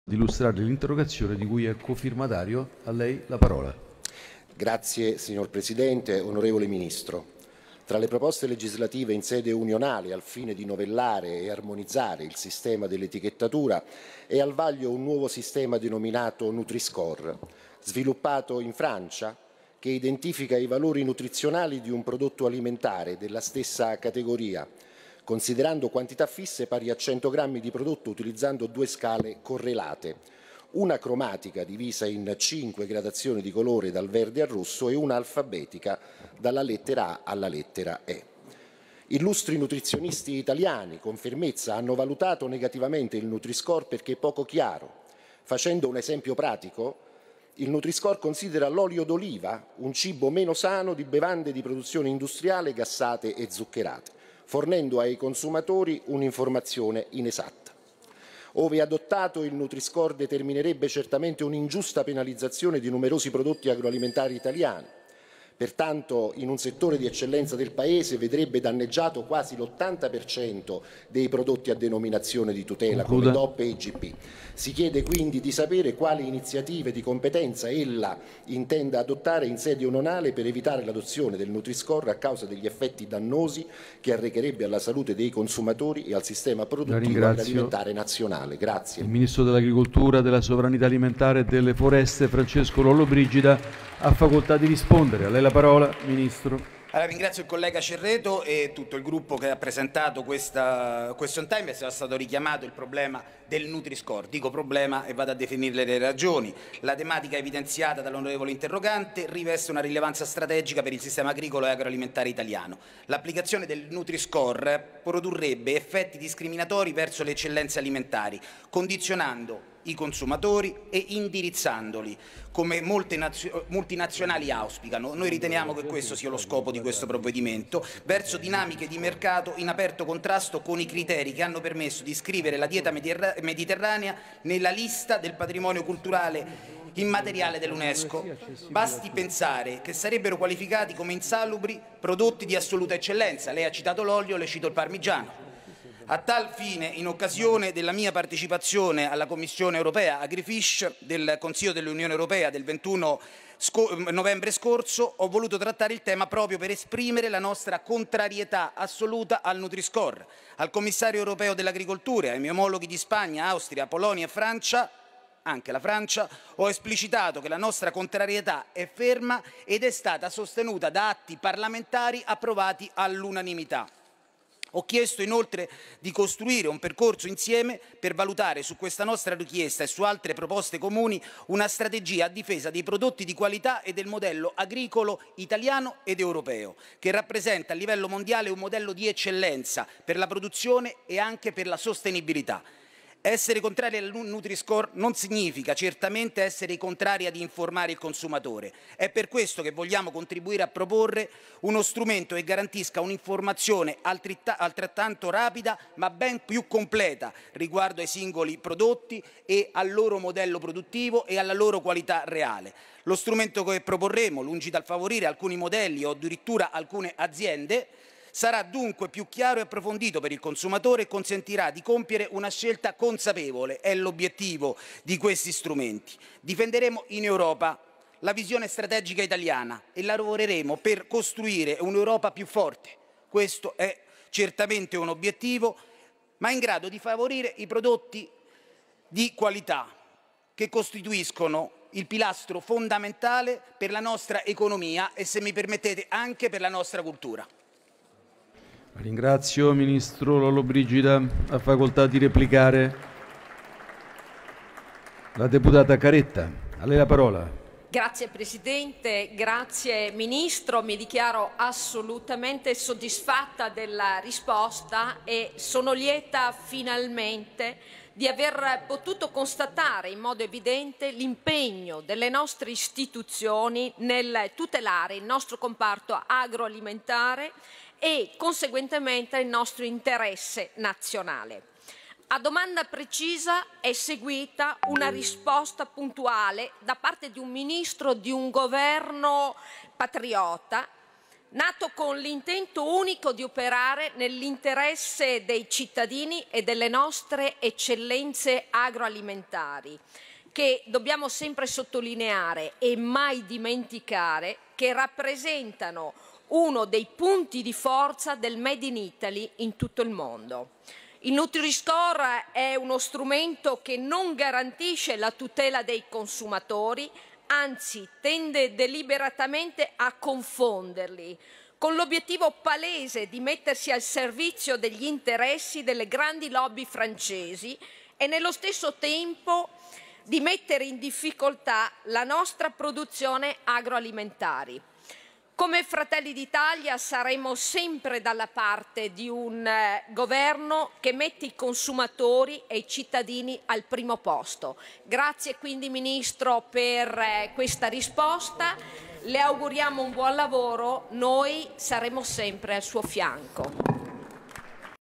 Siamo lieti di constatare impegno delle istituzioni nel tutelare nostro comparto agroalimentare
🔴 SIAMO LIETI FINALMENTE DI CONSTATARE L’IMPEGNO DELLE ISTITUZIONI NEL TUTELARE IL NOSTRO COMPARTO AGROALIMENTARE E IL NOSTRO INTERESSE NAZIONALE

Svolgimento del
Risponde il Ministro dell'agricoltura e della sovranitàalimentare e
Interroga l'On. Marco Cerreto e replica l'On. Maria Cristina Caretta